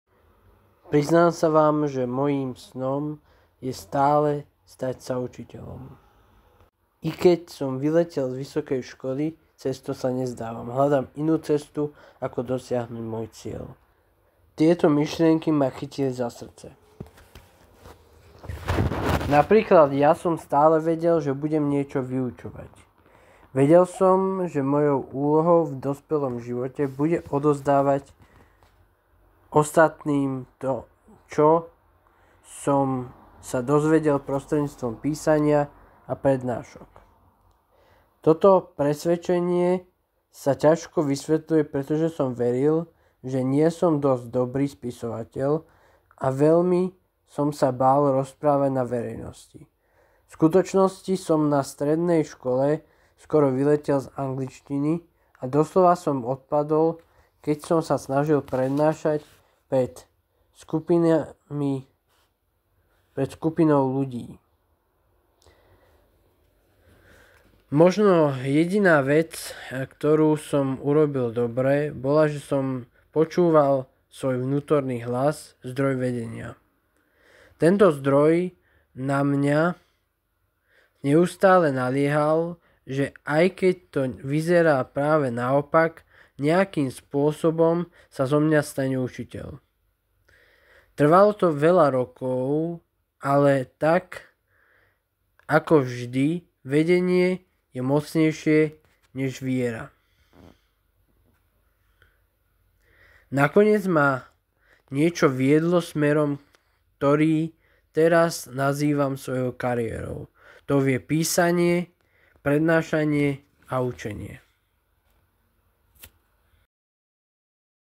Netrápte sa... Po chorobe sa mi otvára šanca robiť to, čo chcem robiť!
Netrápte sa... Po chorobe sa mi otvára šanca robiť to, čo chcem robiť! Mám veľké sny a nechcem sa ich jednoducho pustiť, zbaviť či utratiť. Chcem aby žili, pretože iba tak môžem považovať život za úspešný. Dnes sa s vami podelím o myšlienky, ktoré ma chytili za srdce.